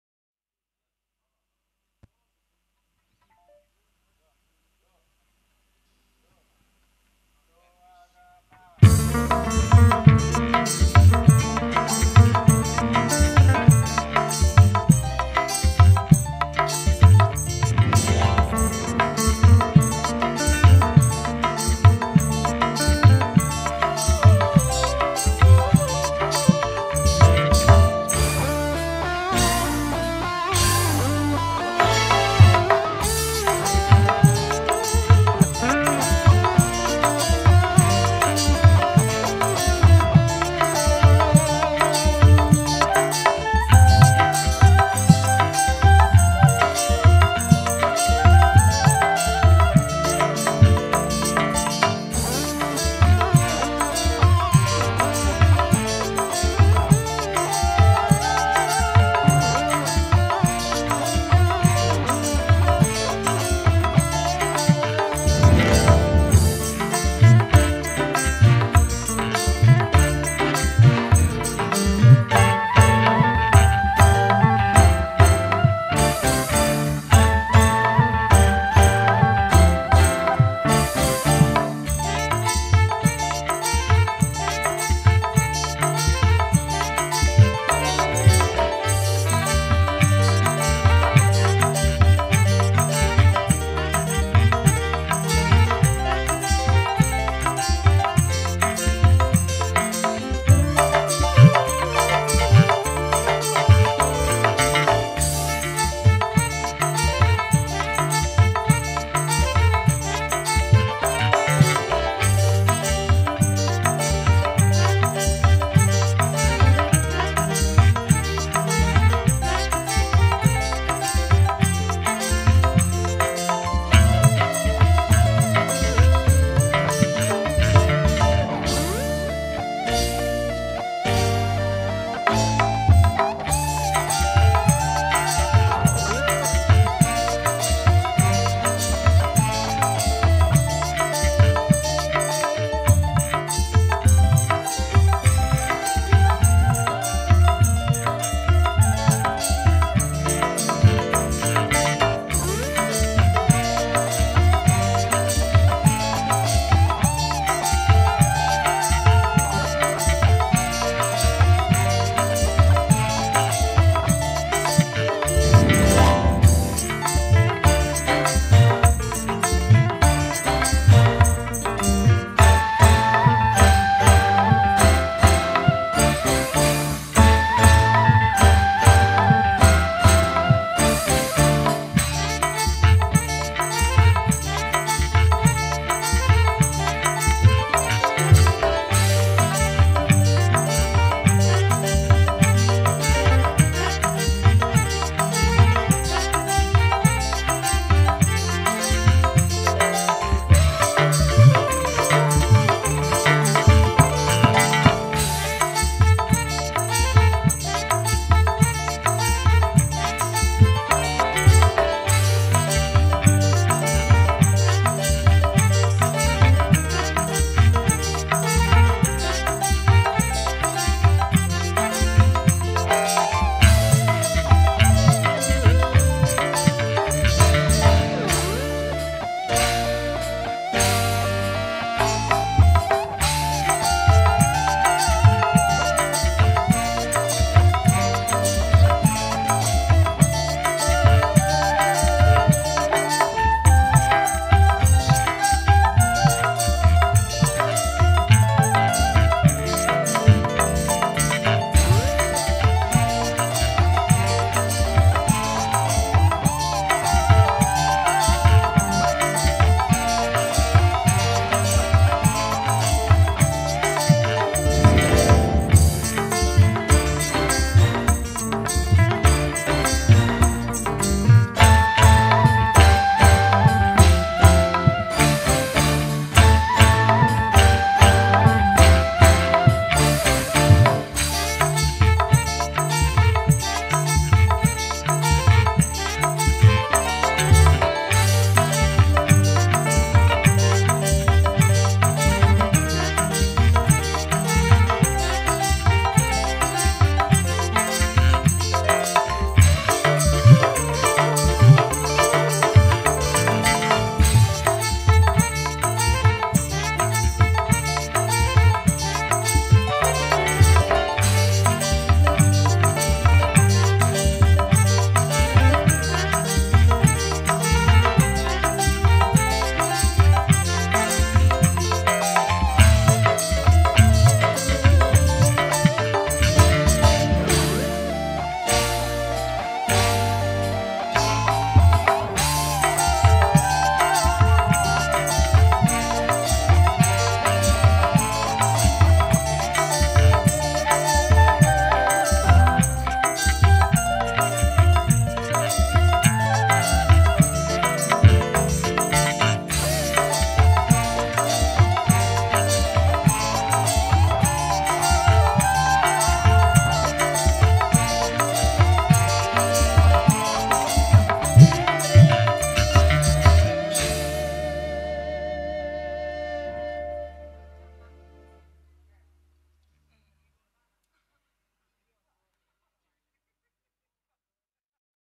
WARLAN MUDA INSTRUMEN
Pesta perhajatan. Bpk. Herman Ibu Cicih // Dalam mengkhitankan putra tercinta. Jang Dicky Al Maher // Pada Hari. Jum'at, 9 April 2021.
Bertempat. Dusun Bondan RT. 020 / 005 Desa. Sidamulya Kec. Cipunagara Subang